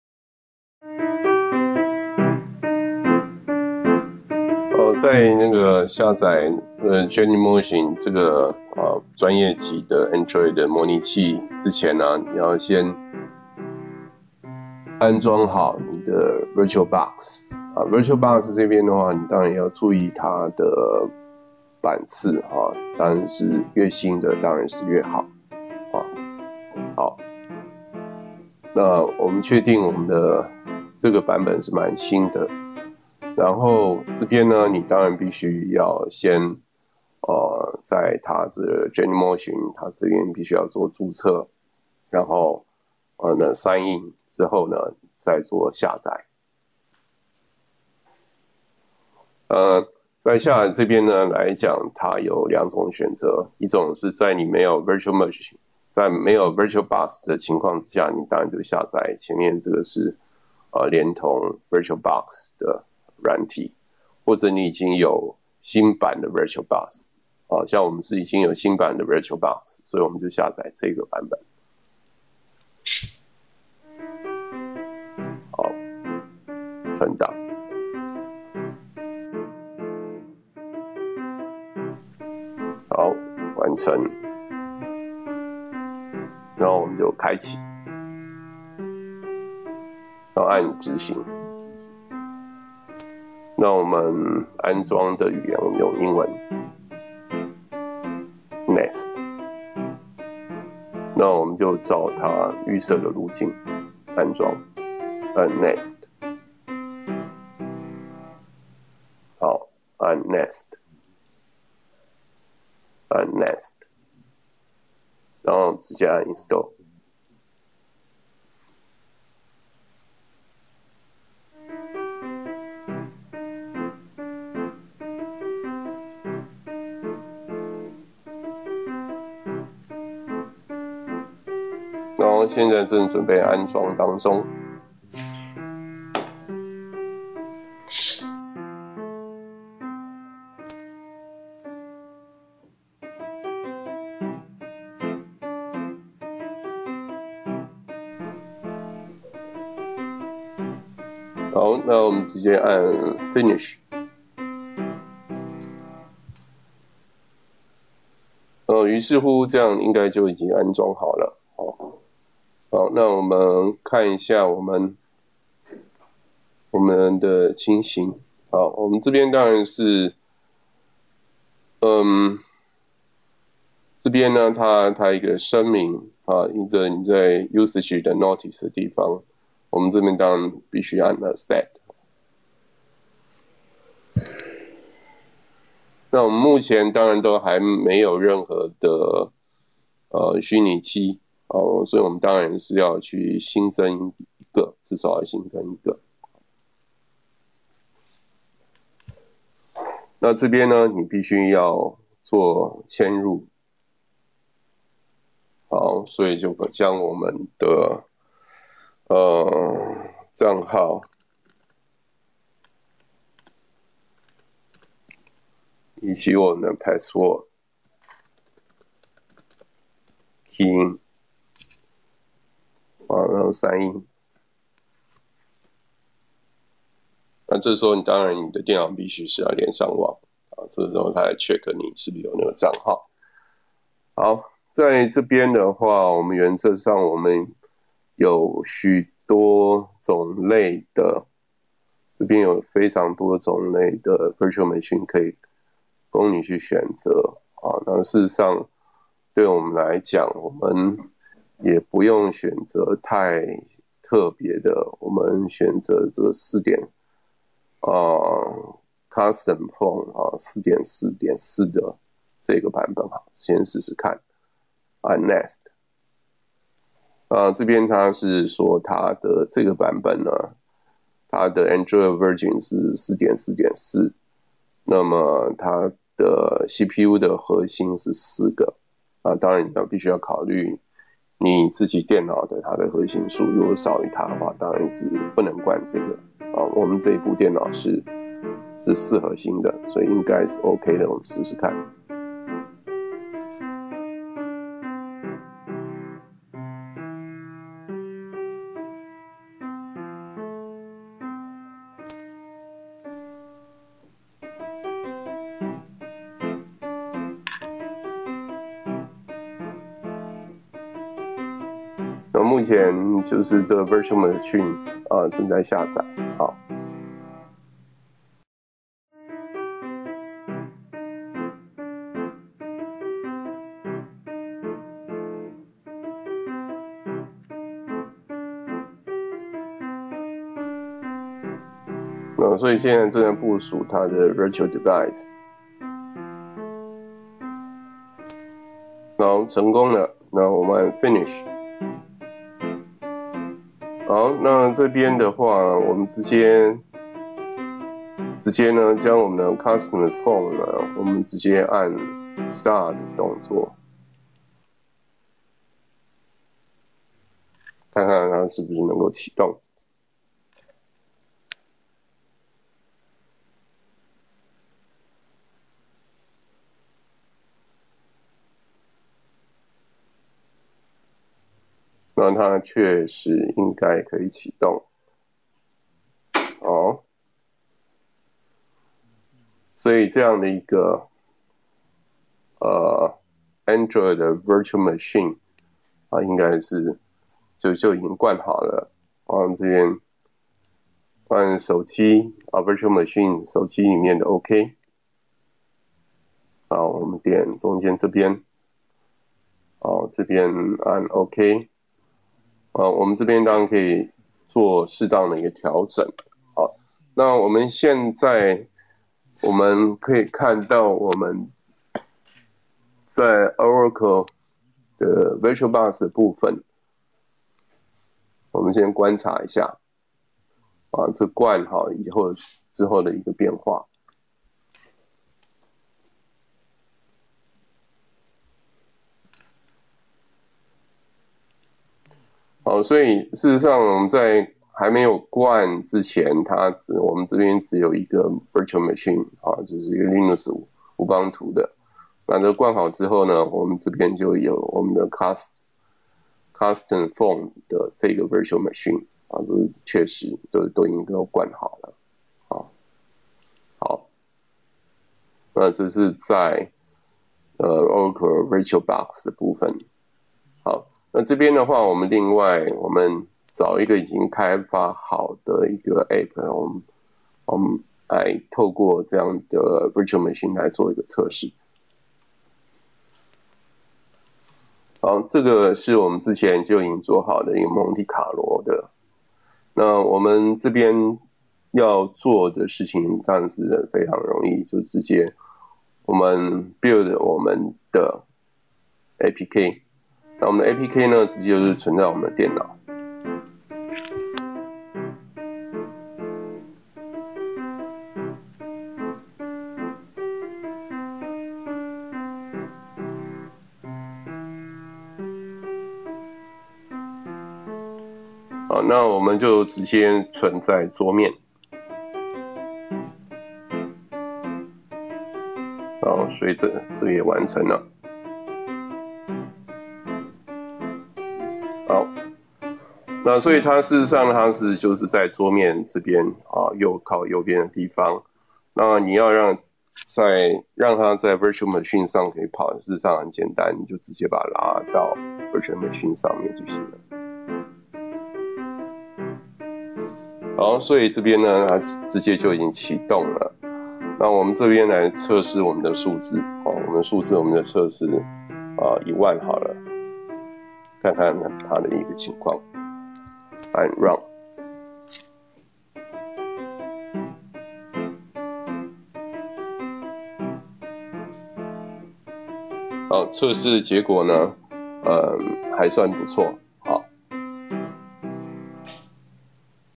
genymotion android虛擬手機的安裝與APP 的測試
genymotion android虛擬手機非常專業，要比eclipse、android studio或是app inventor 2的都優越。利用蒙地卡羅算PI的APP來測試，發現性能非常良好。